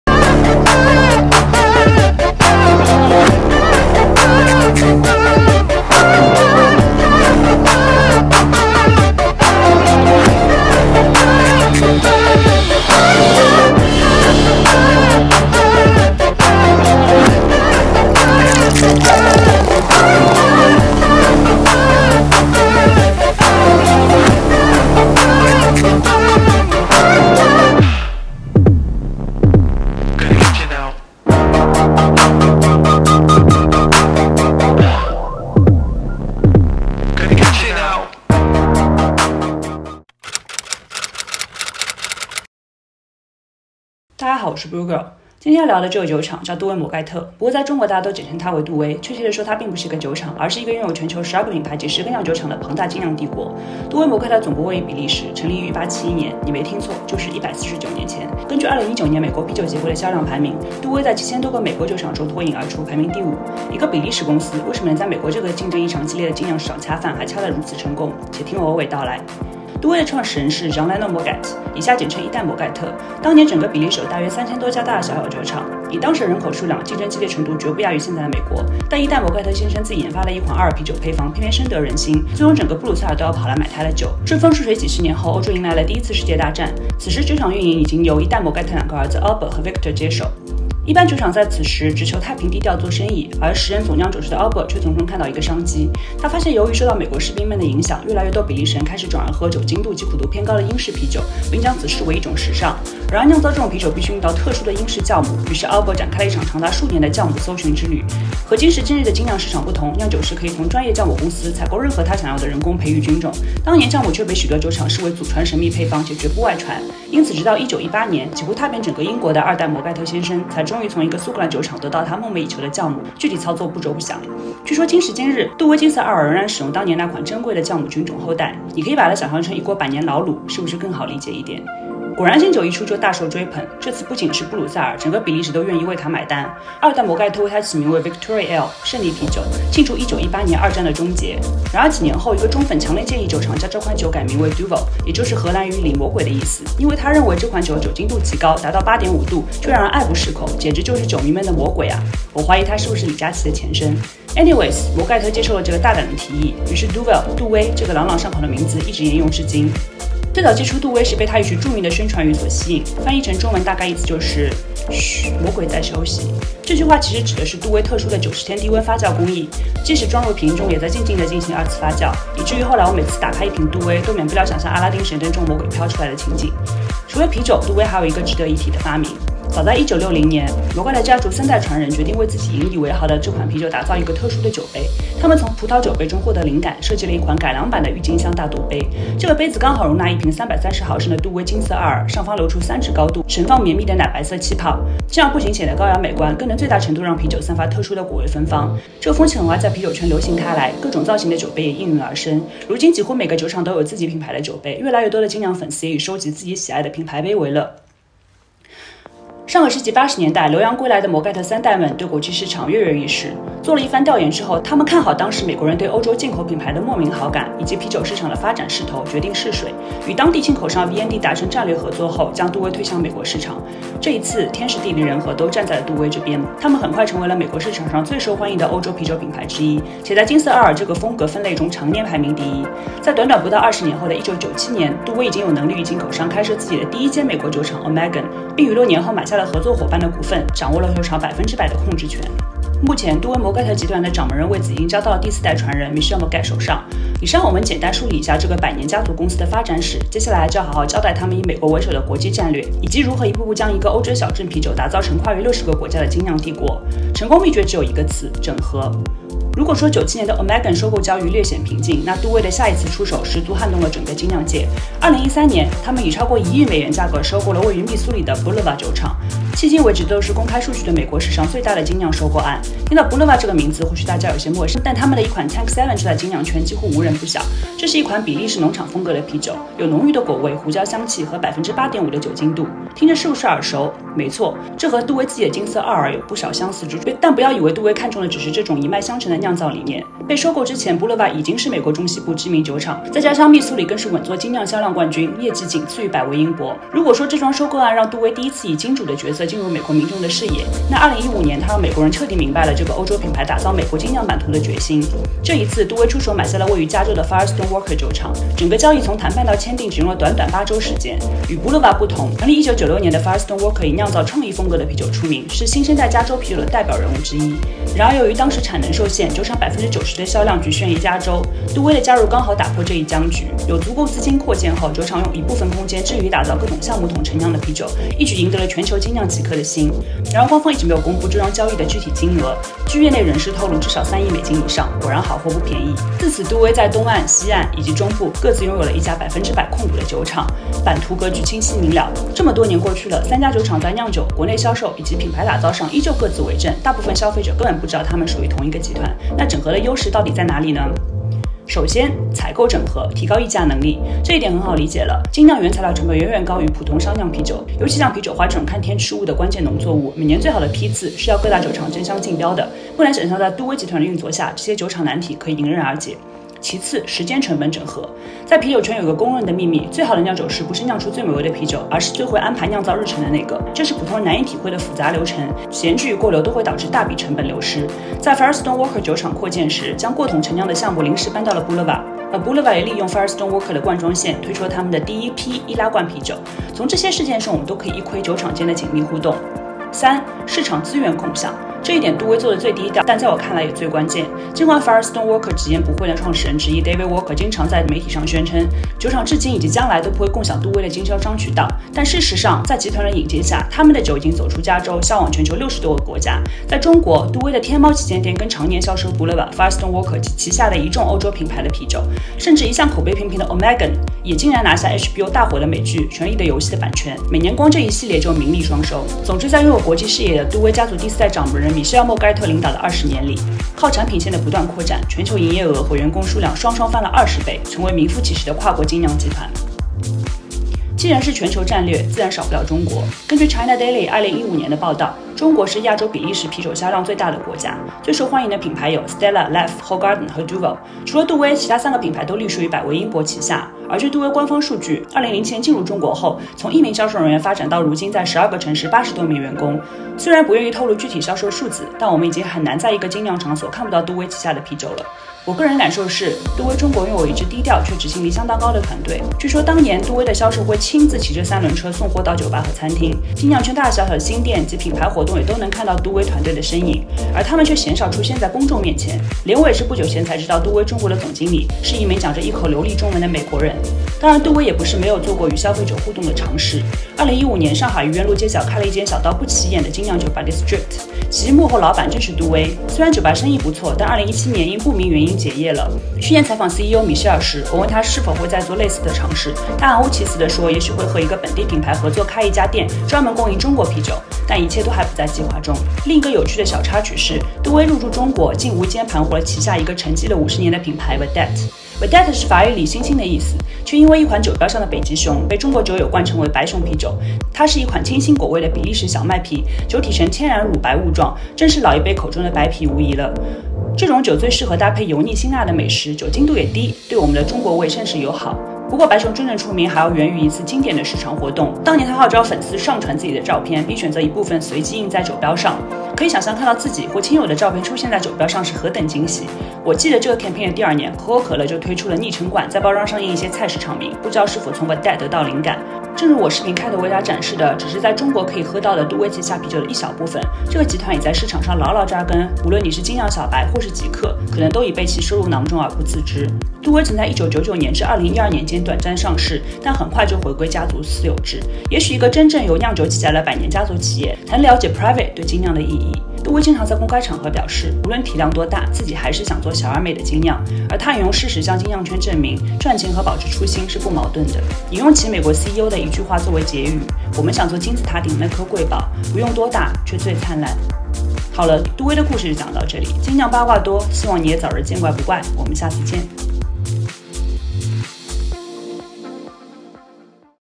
This Craft Beer Brand Knows How to Make Money in China
This story is about Duvel, an often "overlooked" craft beer empire. Since entering China in 2007, nowadays there's barely a craft beer place in China without some beer from the Duvel Moortgat group. However most consumers drink Firestone Walker or Tank 7 by Boulevard without knowing that they belong to a Belgium company. How do they keep such low profile while became one of the most profitable craft beer group in China?